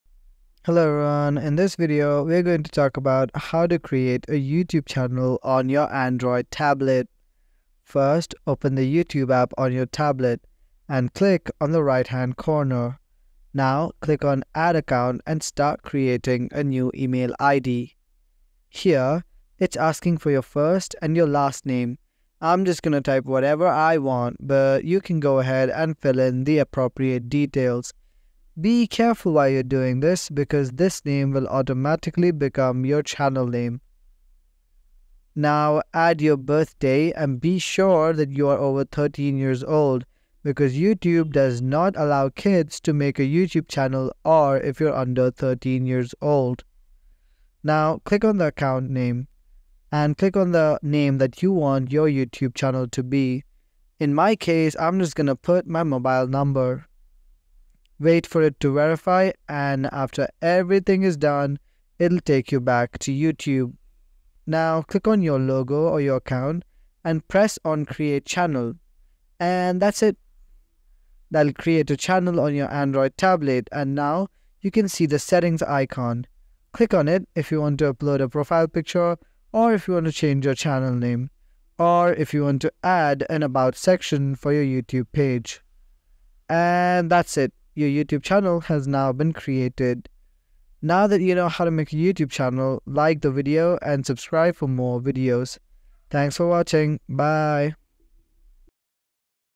How To Make a YouTube Channel on Android Tablet
Unlock the power of YouTube on your Android tablet! In this step-by-step tutorial, discover how to create your very own YouTube channel using your tablet.